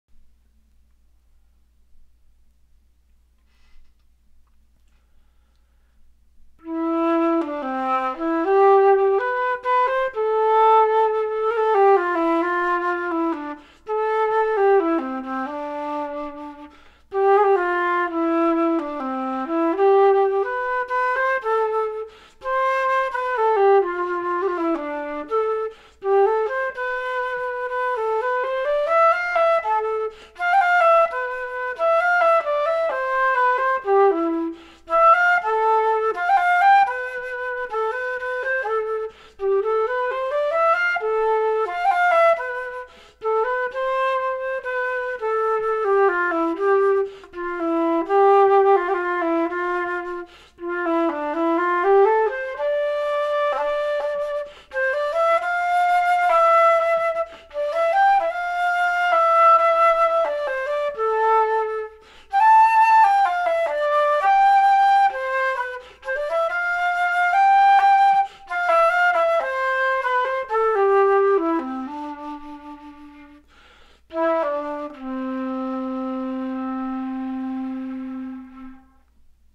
Altus Flute d'Amore.
Check out awesome Flute d'Amore. Love this one. Recording a project with it.